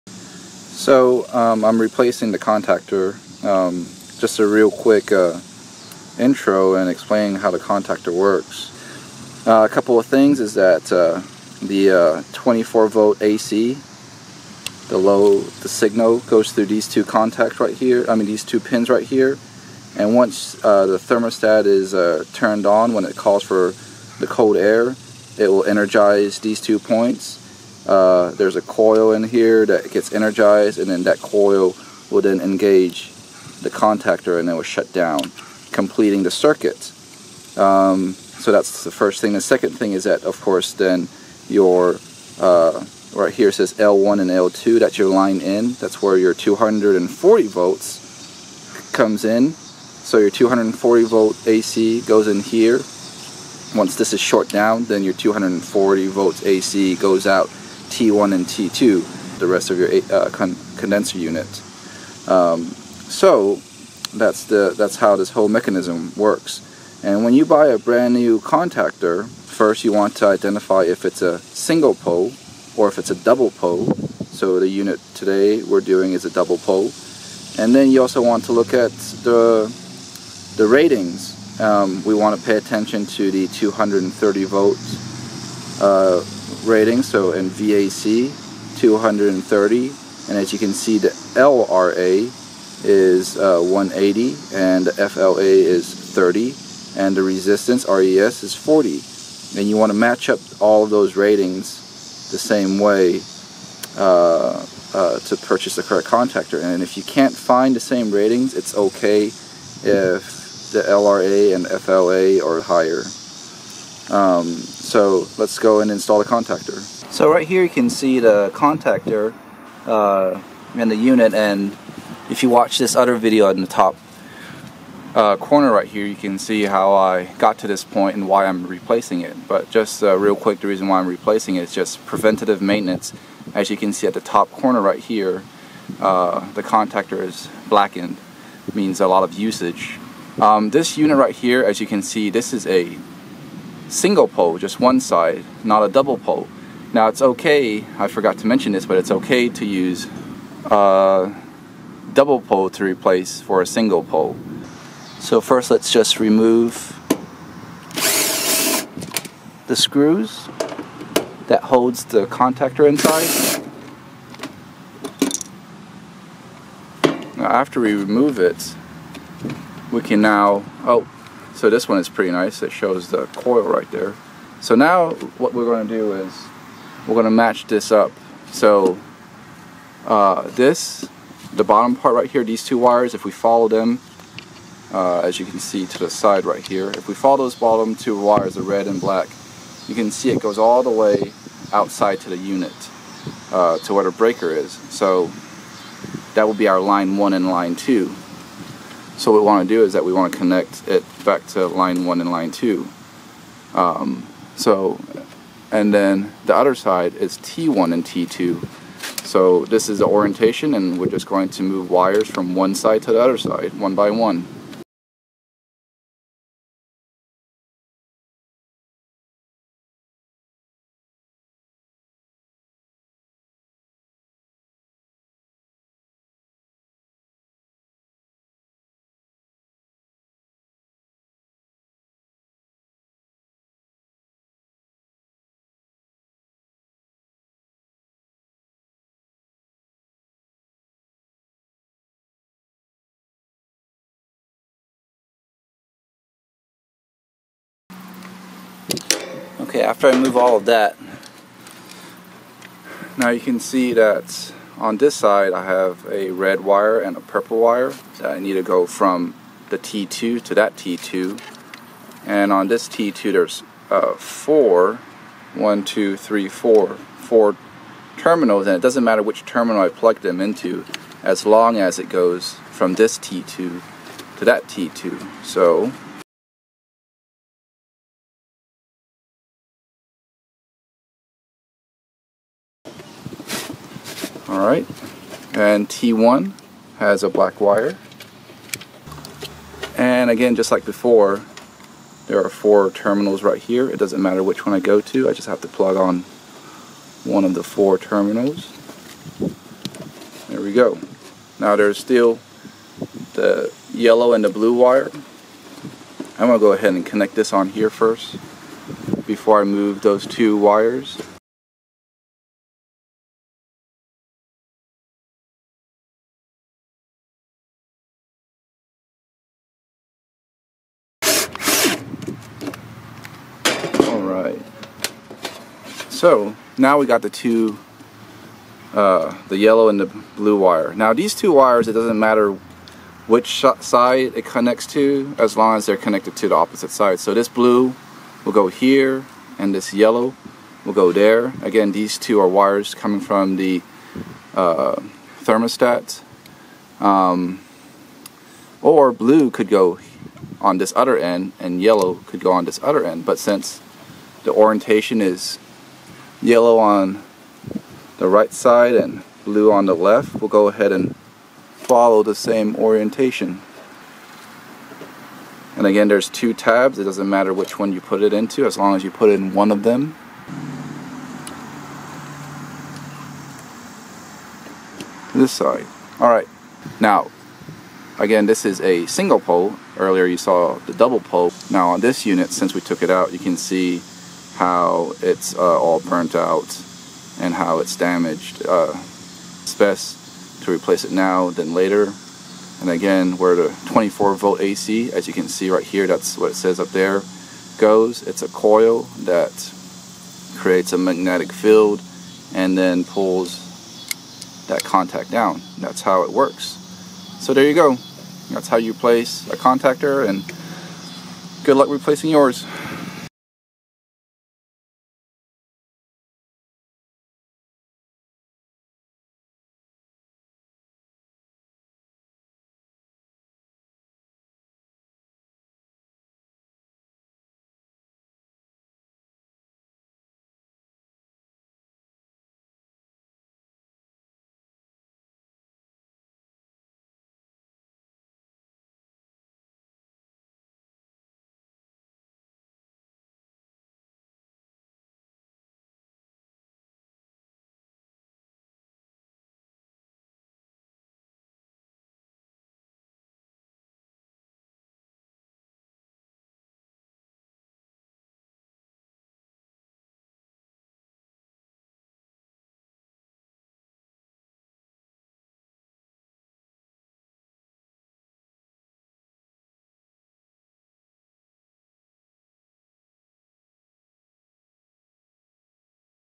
Air condition condenser doesn't Power up - How To Replace a Contactor - HVAC not cooling on
Amazon Review

I am not a professional. I am only documenting what i've done for reference only.